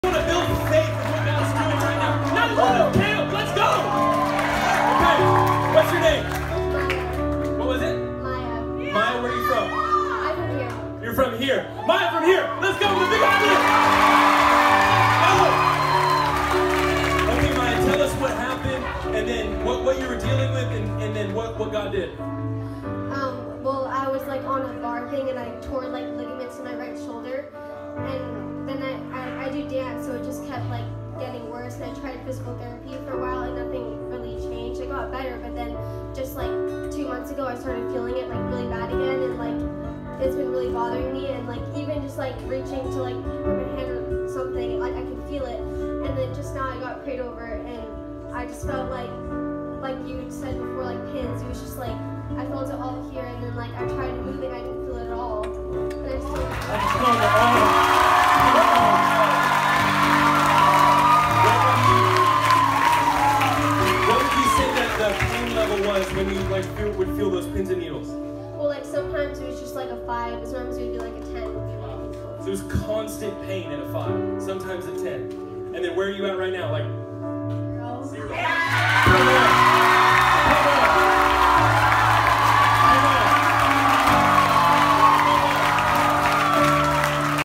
YWAM Kona Youth Commission Camp 2022 Healing Testimony 2
Welcome to New Hope Aloha Pau'ole. Come and be a part of God's Story! Here's another quick testimony of the healing God was doing on the first night! Call out to Jesus if you need healing today!